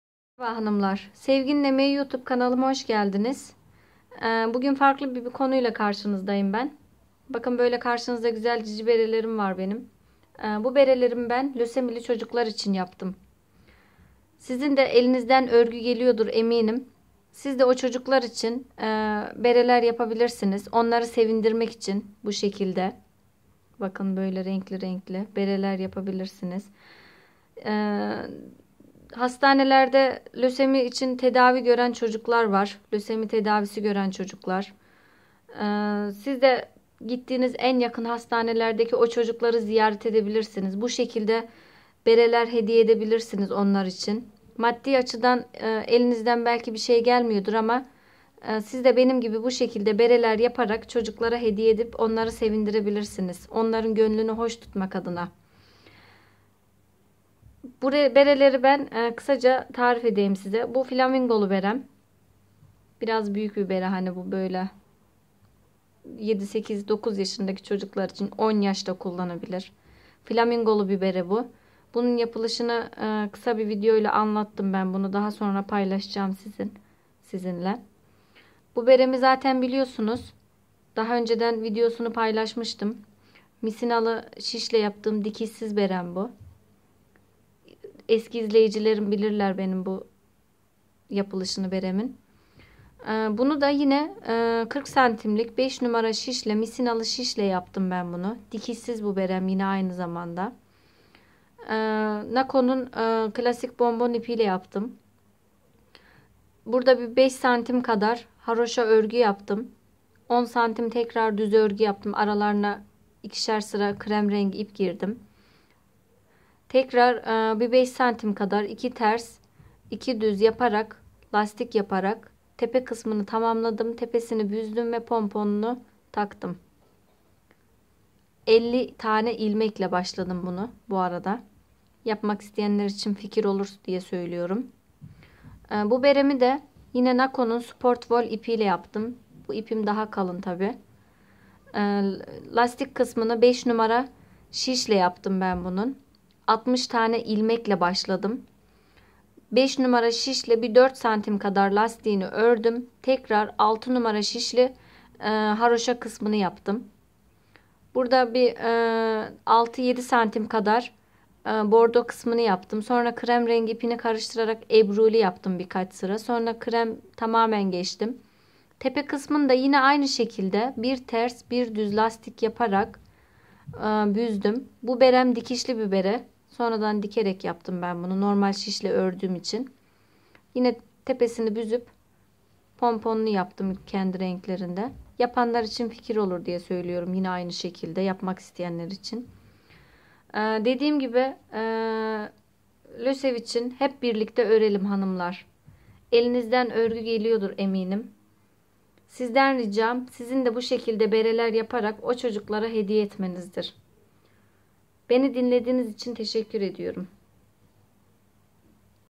Bere Modeli ve Yapılışları - Lösev için
Lösev için .. Elinden örgü gelen örgü yapmayı bilen hanımlar hepinizden ricam lösemi tedavisi gören çocuklar için hep beraber örelim..